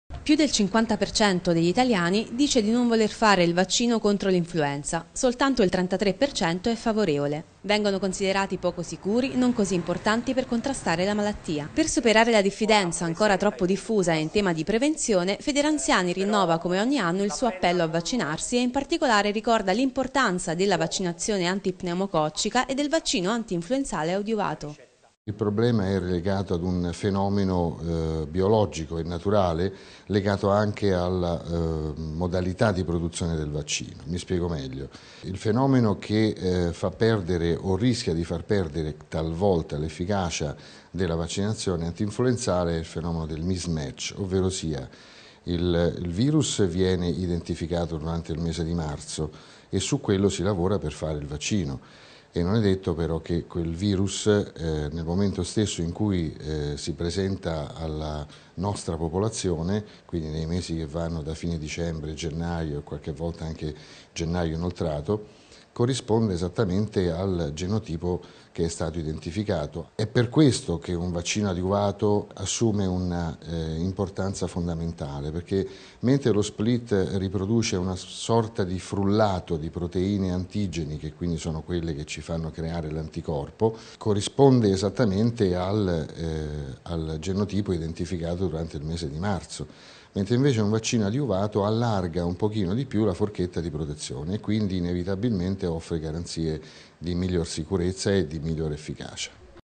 Family Tg 15/10/2013 - L'importanza del vaccino adiuvato
Più del 50 per cento degli italiani dice di non voler fare il vaccino contro l'influenza: sono considerati poco sicuri. Per superare la diffidenza in tema di prevenzione Federanziani rinnova come ogni anno il suo appello a vaccinarsi e in particolare ricorda l'importanza del vaccino antiinfluenzale adiuvato. Dei suoi vantaggi ce ne ha parlato Luigi Sudano, comitato scientifico FederAnziani.